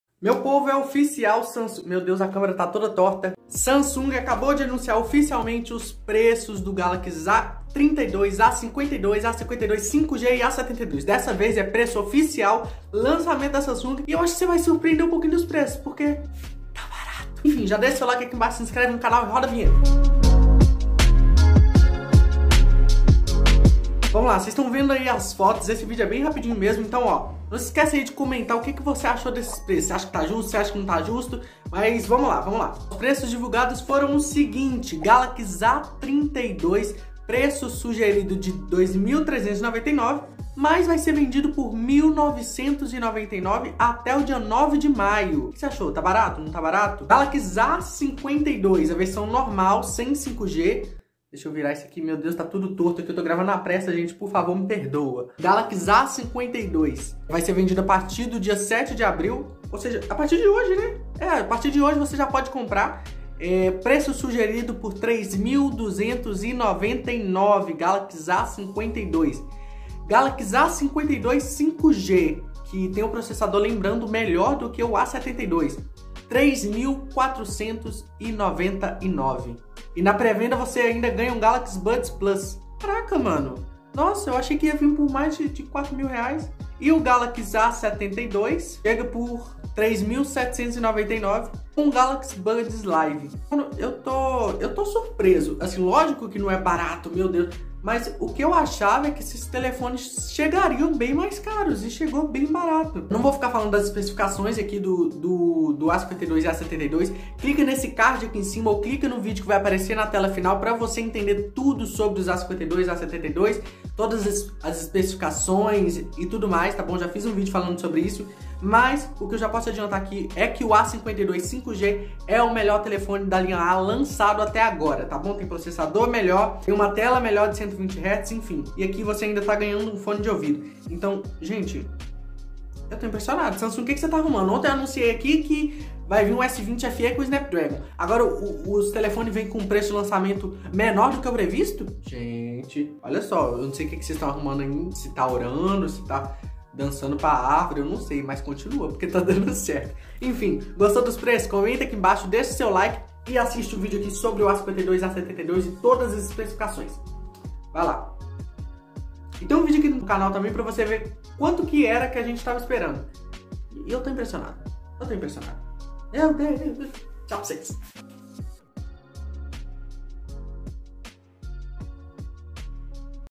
SAIU!! Preço OFICIAL Galaxy A32, A52, A52 5G e A72
Saiu os preços oficiais dos novos Galaxy A32, A52, A52 5G e A72 pro Brasil... Você vai ficar surpreso!!!
Ja diz ai, o que achou dos preços, e quanto você acha que eles deveriam custar?